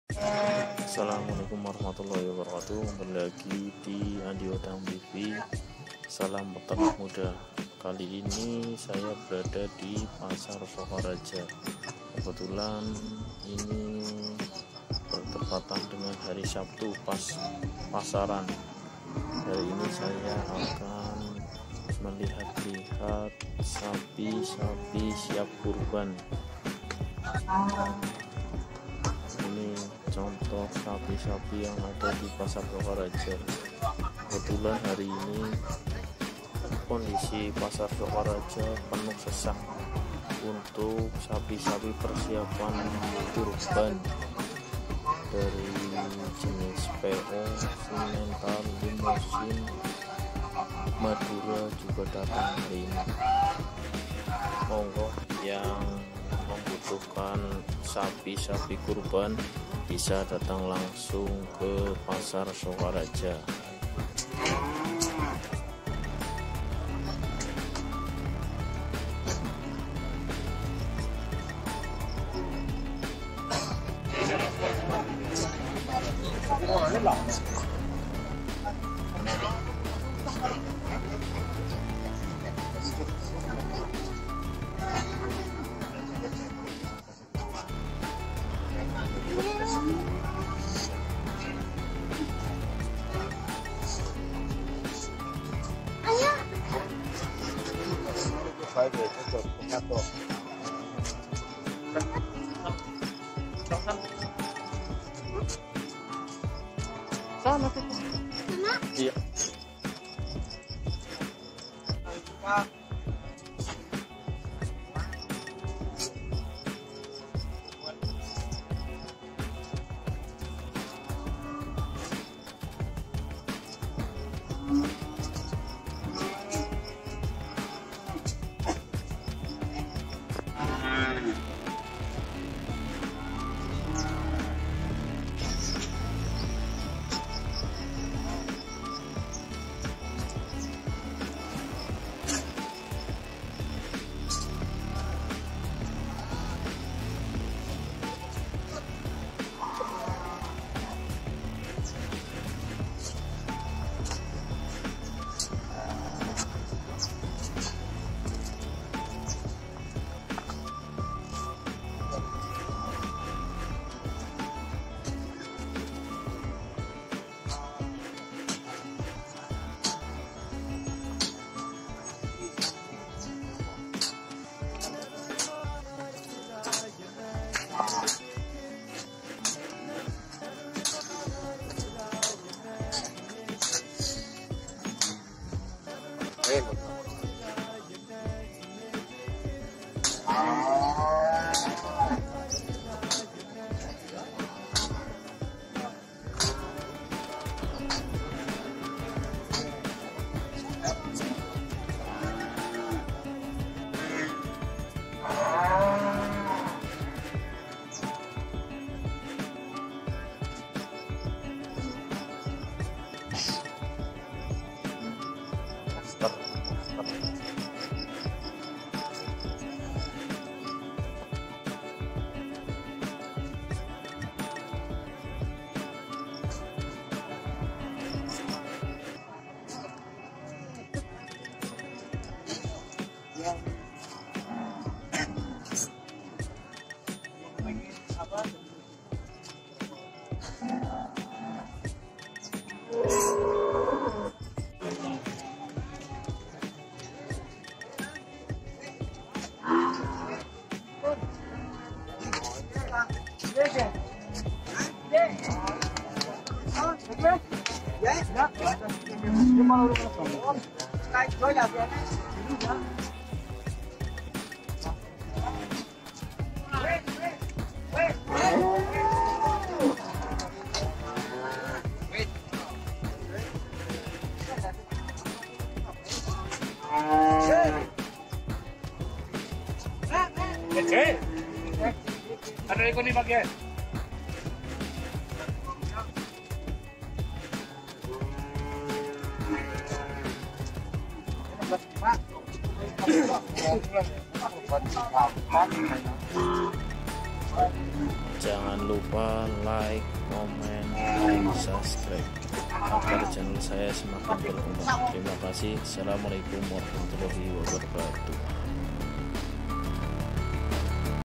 sapi siap kurban di pasar Sokaraja
sapi-sapi siap kurban di pasar Sokaraja kali ini membludak,harga mulai dari 18jtan tergantung ukuran dan jenis#AndiodankTV#sapi#bakulsapi